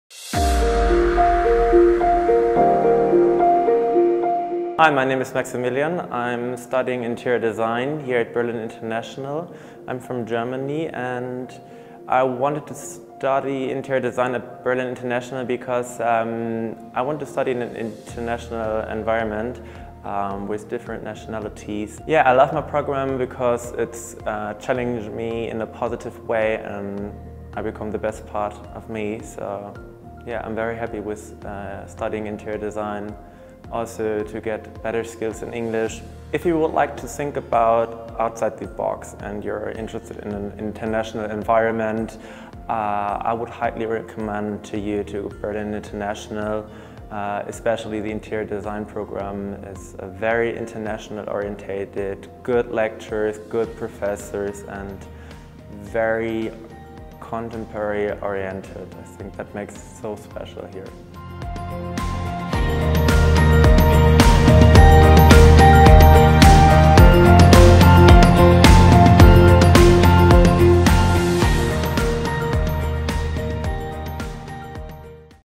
Maximilian Arkenau about Berlin International University of Applied Sciences!
Enjoy the short talk of our Interior Design student about how studying at Berlin International is like!